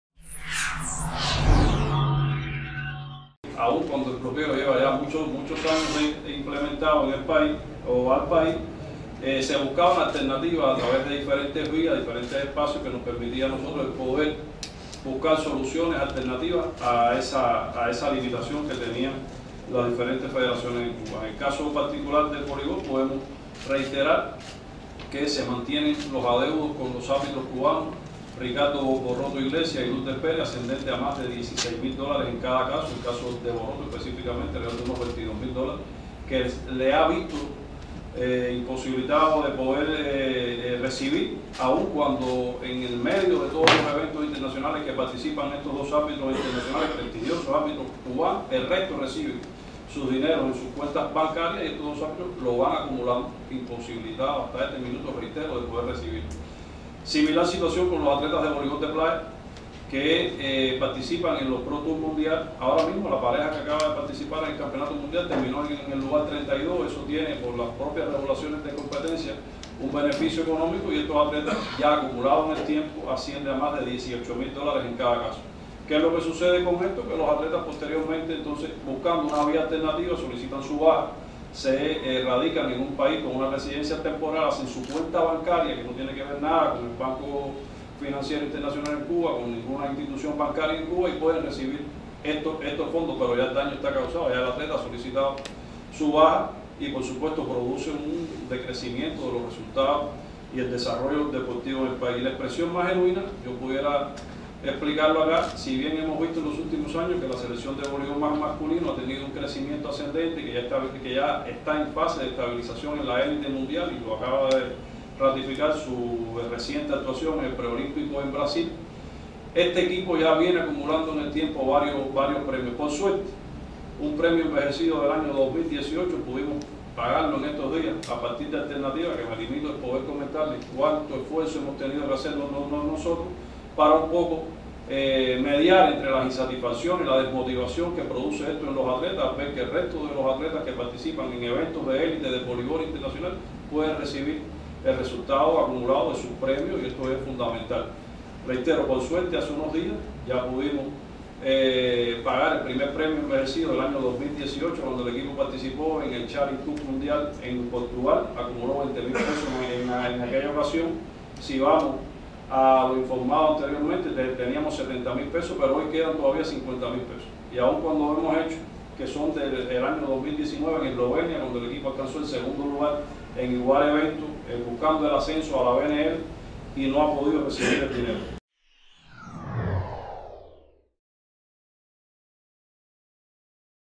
Ariel Sainz, vicepresidente del Inder sobre afectaciones del bloqueo al Voleibol
Para Ariel Sainz, vicepresidente del Inder y titular de la Federación
Cubana de Voleibol, es claro que su deporte pudiera contar con mucho
más desarrollo de poder situarse en condiciones de igualdad con el
resto de las entidades del orbe.